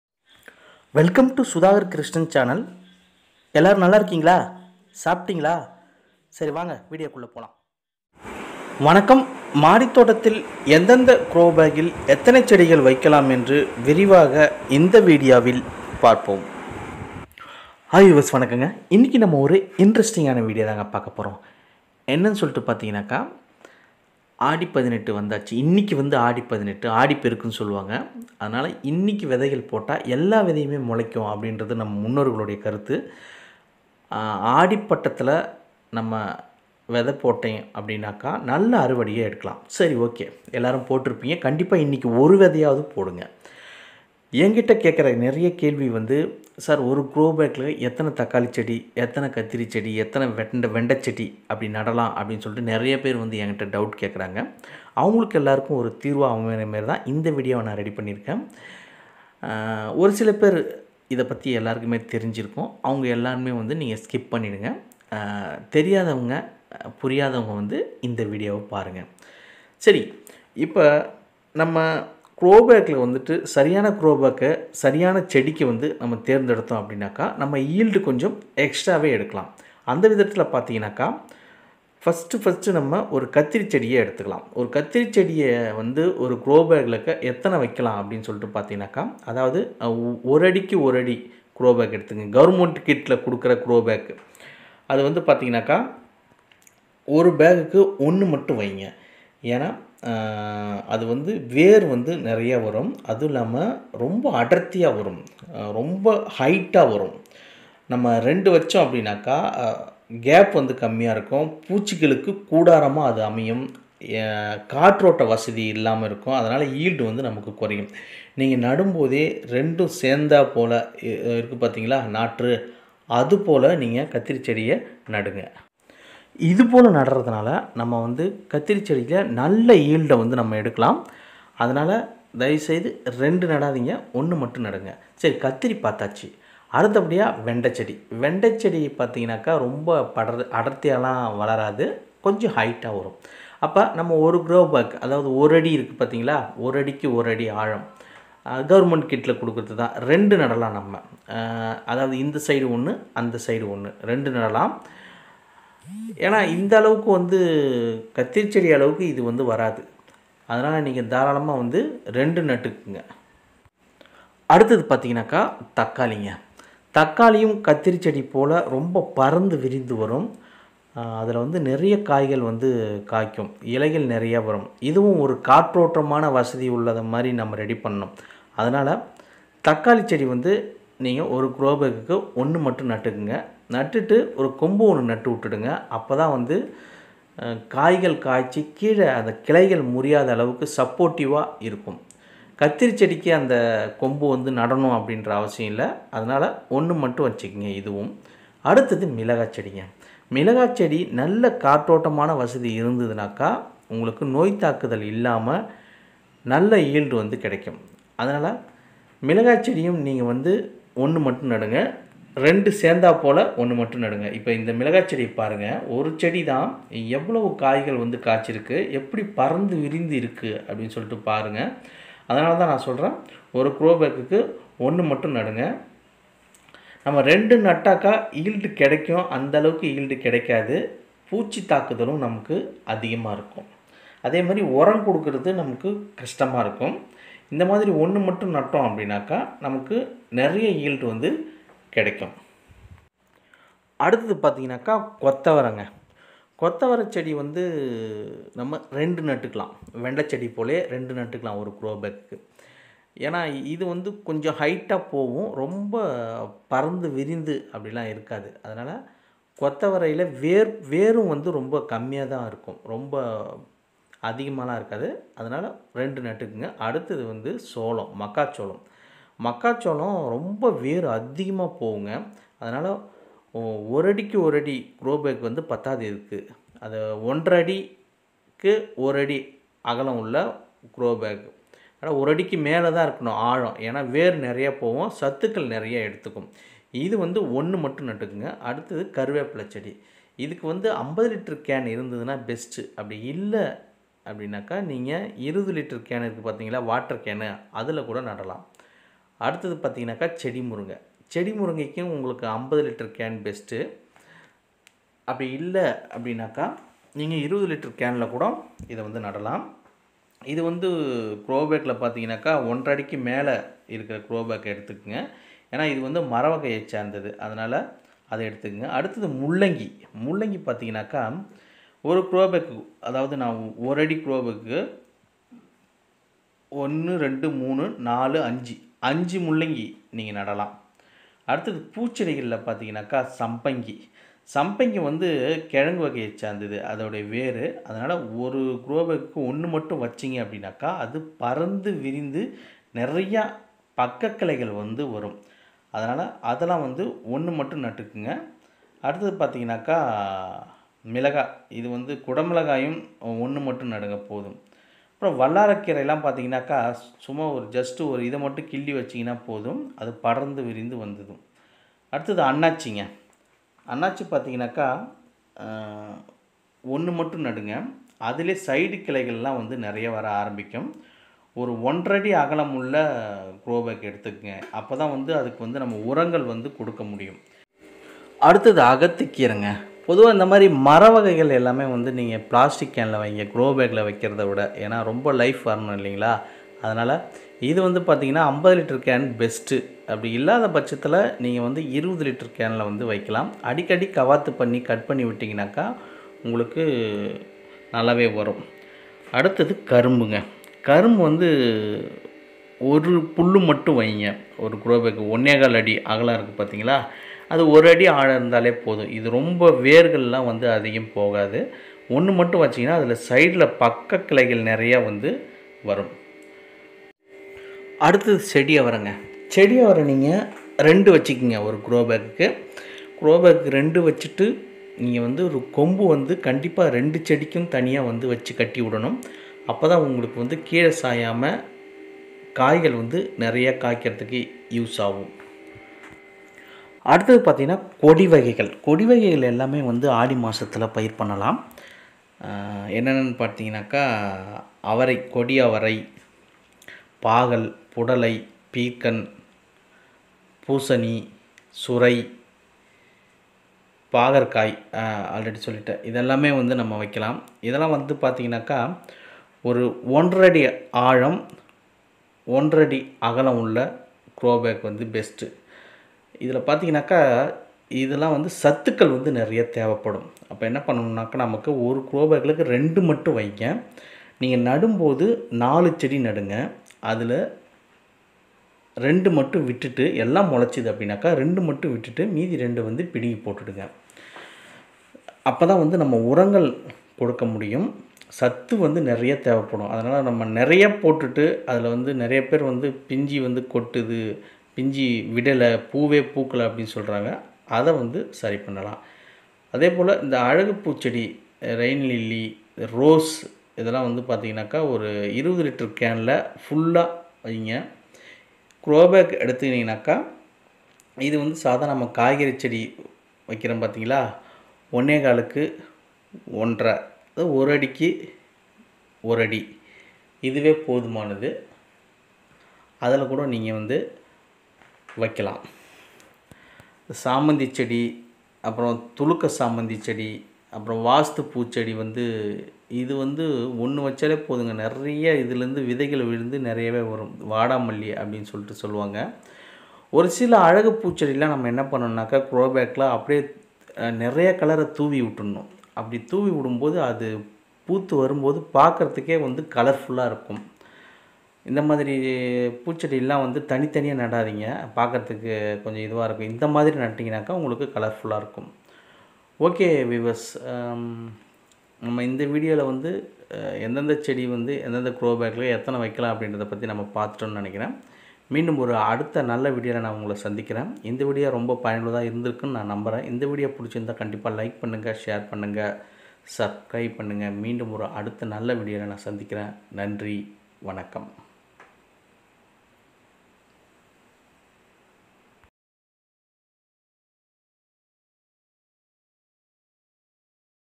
மாடி தோட்டத்தில் செடி, கொடிகளை grow bag ல் இது போல் வைத்து பாருங்கள் தாறுமாறான விளைச்சலை எடுக்கலாம்!
sudagarkrishnan மாடி தோட்டத்தில் எந்த எந்த grow bag ல் எத்தனை செடிகள் வைத்தால் நல்ல விளைச்சலை எடுக்கலாம் என்று விரிவாக இந்த வீடியோவில் பார்ப்போம்.